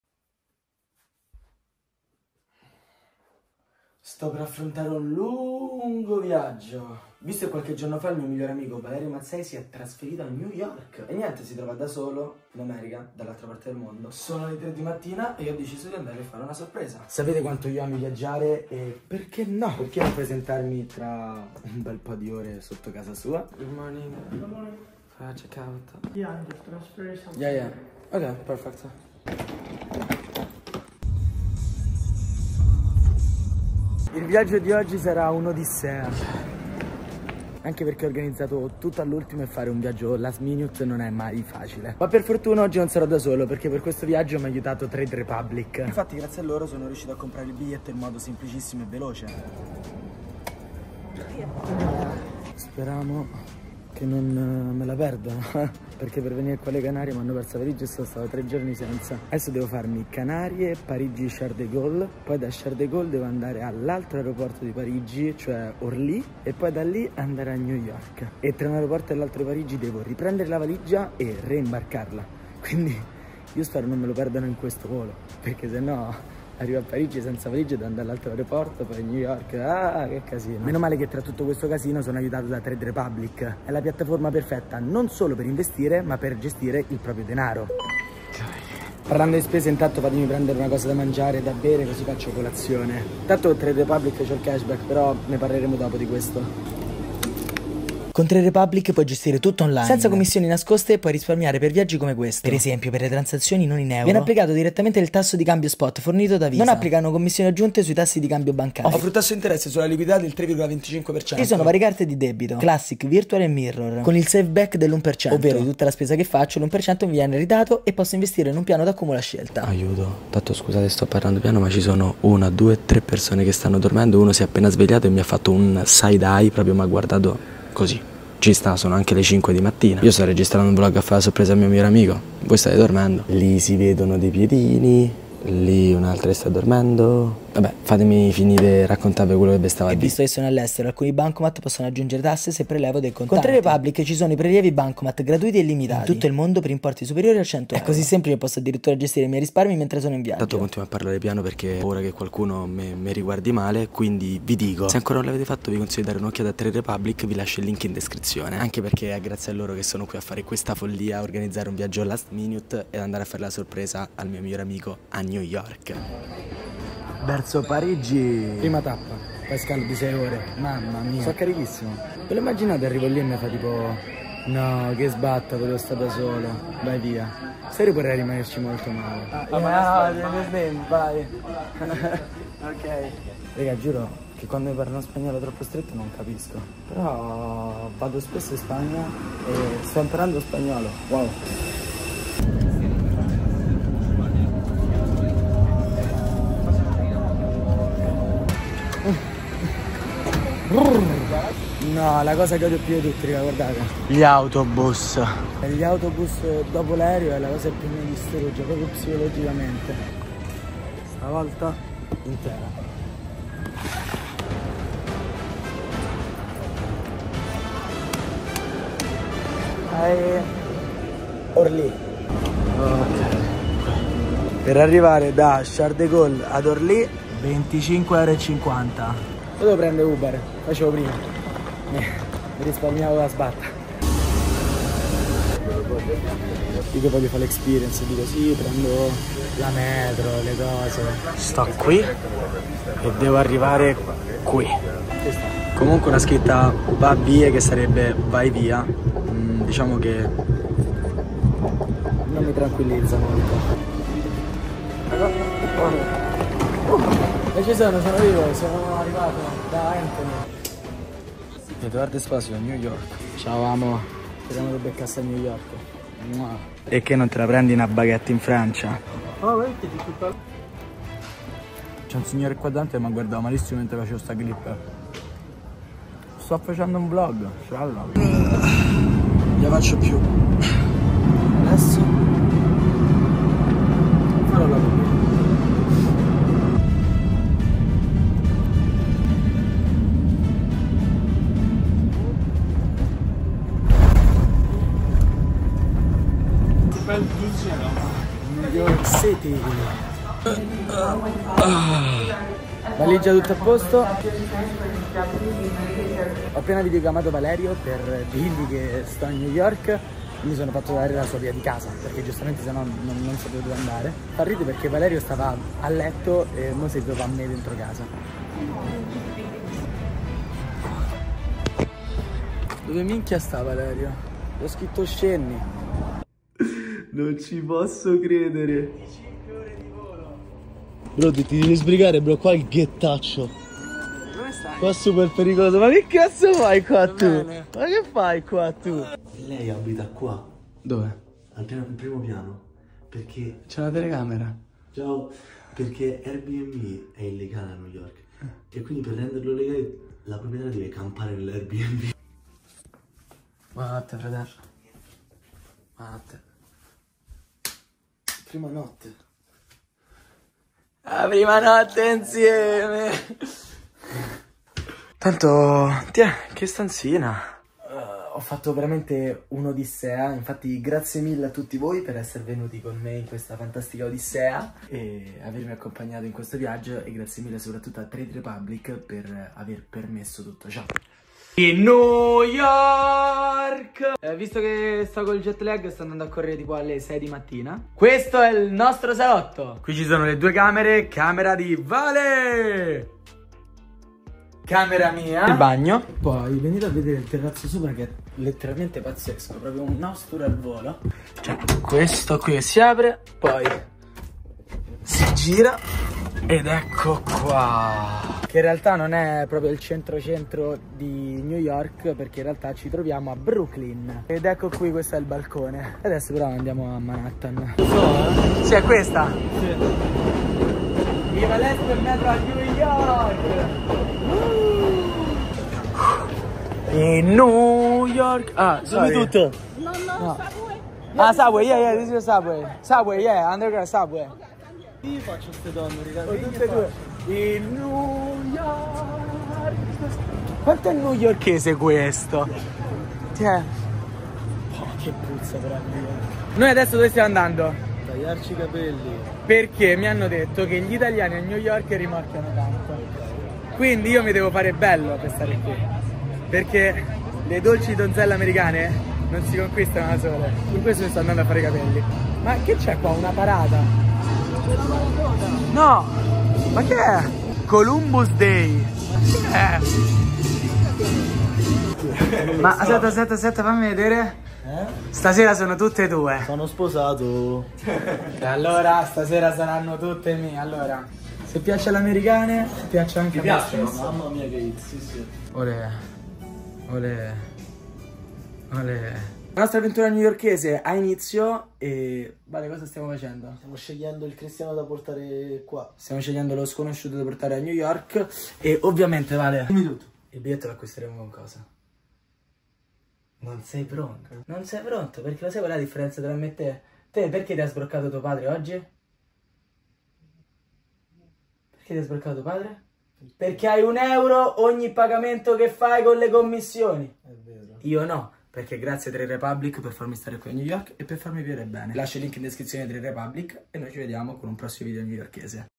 Sorprendo Il Mio Migliore Amico A New York
Visto che Valerio Mazzei è andato a vivere a New York, ho deciso di sorprenderlo, andare da lui e fargli compagnia